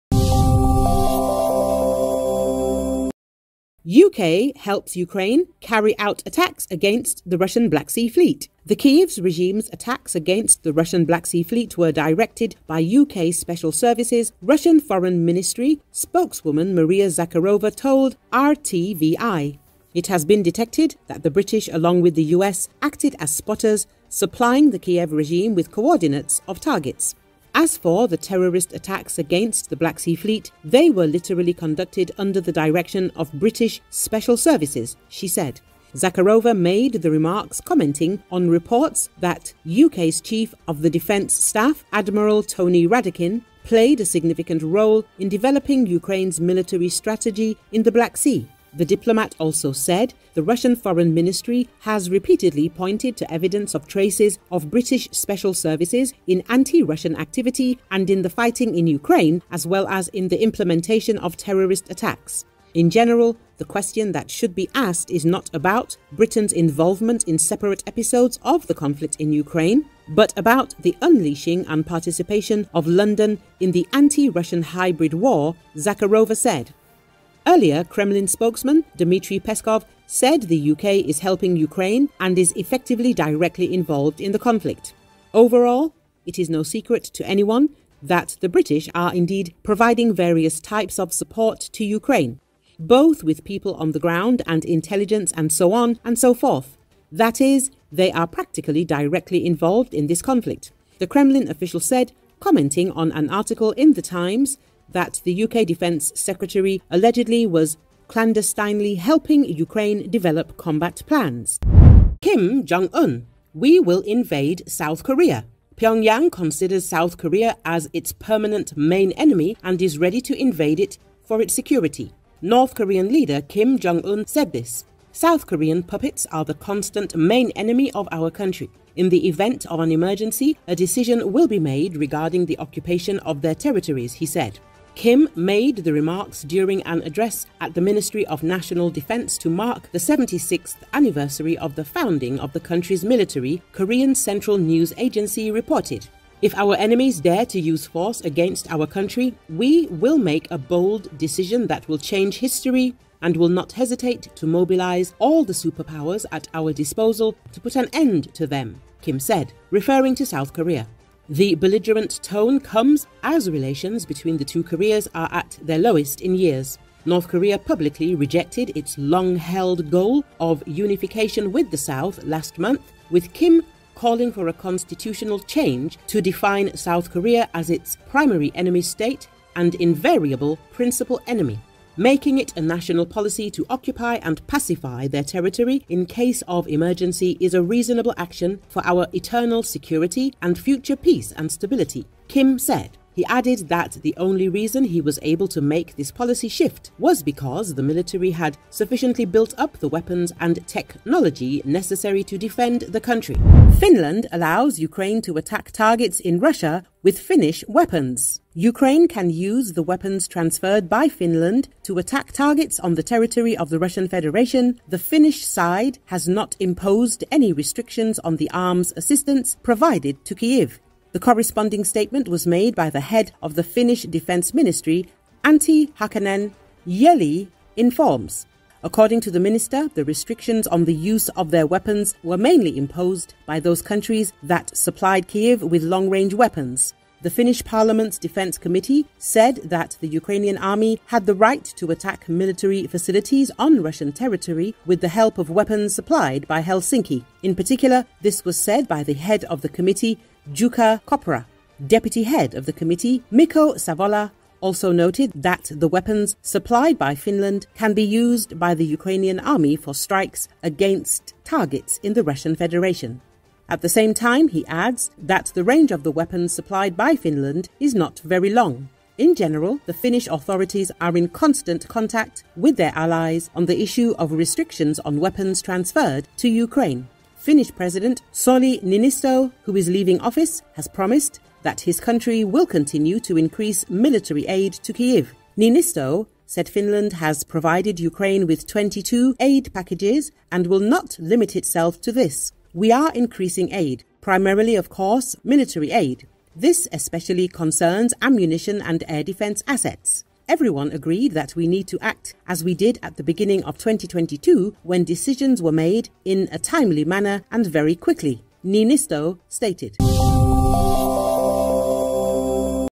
UK helps Ukraine carry out attacks against the Russian Black Sea Fleet - Kremlin claims
The Kiev regime's attacks against the Russian Black Sea Fleet were directed by UK special services, Russian Foreign Ministry Spokeswoman Maria Zakharova told RTVI.
"It has been detected that the British, along with the US, acted as spotters, supplying the Kiev regime with coordinates of targets. As for the terrorist attacks against the Black Sea Fleet, they were literally conducted under the direction of British special services," she said.
Zakharova made the remarks commenting on reports that UK’s Chief of the Defense Staff Admiral Tony Radakin played a significant role in developing Ukraine's military strategy in the Black Sea.
The diplomat also said the Russian Foreign Ministry "has repeatedly pointed to evidence of traces of British special services in anti-Russian activity and in the fighting in Ukraine, as well as in the implementation of terrorist attacks." "In general, the question that should be asked is not about Britain's involvement in separate episodes of the conflict in Ukraine, but about the unleashing and participation of London in the anti-Russian hybrid war," Zakharova said.
Earlier, Kremlin Spokesman Dmitriy Peskov said the UK is helping Ukraine and is effectively directly involved in the conflict.
"Overall, it is no secret to anyone that the British are indeed providing various types of support to Ukraine. Both with people on the ground, and intelligence, and so on, and so forth. That is, they are practically directly involved in this conflict," the Kremlin official said, commenting on an article in the Times that the UK defense secretary allegedly was clandestinely helping Ukraine develop combat plans.